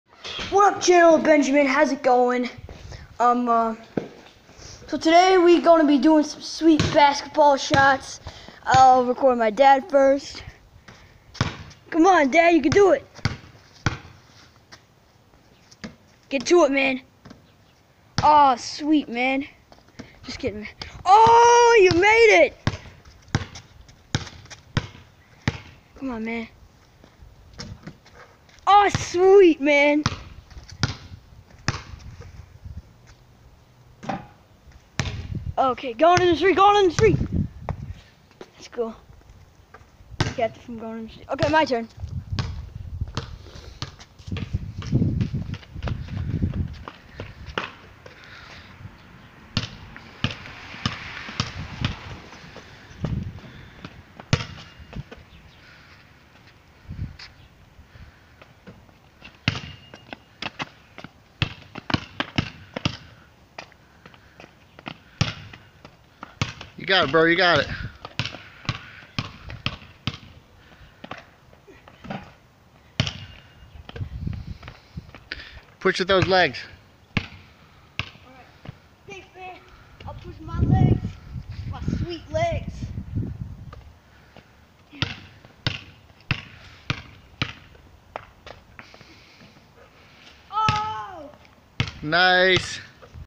Sweet 🏀 shots
So Benjamin and Dad were gonna play some 🏀 and we got it on tape